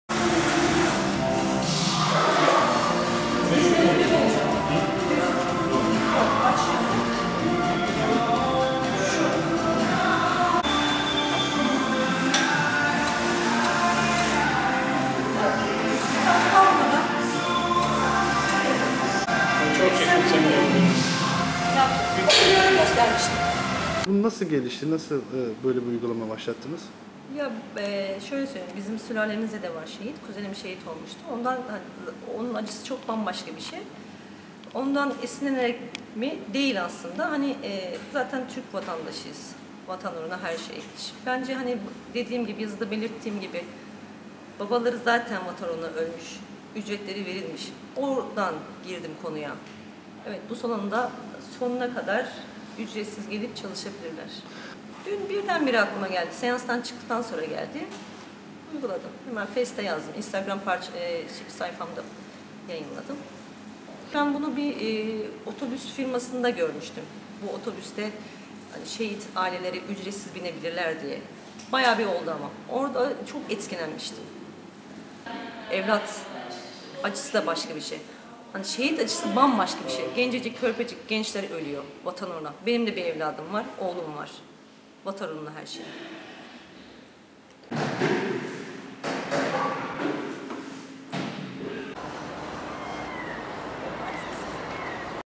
MEHTAP SPOR CENTER
BU SALON, ŞEHİT ÇOCUKLARINA ÜCRETSİZ
EREĞLİ
Zonguldak’ın Ereğli ilçesinde spor salonu işletmecisi Mehtap Sümer Asil, vatan toprağına düşen şehit çocukları için ücretsiz uygulama başlattığını bildirdi.
Asil, Müftü Mahallesi Filtepe Sokak’ta kendi ismini taşıyan Mehtap Spor Center’da Hakimiyet’e açıklamasında, uygulamadan yararlanmak isteyen şehit çocuklarının salon ücretlerinin, babaları tarafından önendiğini söyledi. Asker kuzeninin yaklaşık 20 yıl önce şehit düştüğünü, kendisinin de bir oğlu olduğunu, vatan uğruna ise her şeyin yapılması gerektiğini anlatan Asil, şöyle dedi:
“Sülalemde de şehidimiz var, kuzenim yıllar önce şehit olmuştu. Bunun acısı bambaşka bir şey. Bu uygulamayı, şehit kuzenimden esinlenerek yapmadım ancak Türk vatandaşıyız, vatan uğruna her şey yaparız. Bu durum, dün seanstan çıktıktan sonra birden aklıma geldi. Aslında buna benzer uygulamayı bir otobüs firmasında görmüştüm. ‘Şehit aileleri ücretsiz binebilirler’ yazıyordu. Bundan çok etkilenmiştim.
Spor salonuma gelen şehit çocukları, burada ücretsiz spor yapabilirler. Paraları, vatan uğruna şehit olan babaları tarafından ödenmiştir. Bu salonda, sonuna kadar ücret vermeden çalışabilirler.
Evlat acısı başka bir şey ancak şehit acısı bambaşka bir şeydir. Gencecik, körpecik gençler toprağa düşüyor. Benimde bir evladım var, her şey vatan uğrunadır.”
Son günlerde gelen şehit haberleri ile adeta yıkıldığını ifade eden Mehtap Sümer Asil, terörün bitirilmesini istediğini sözlerine ekledi.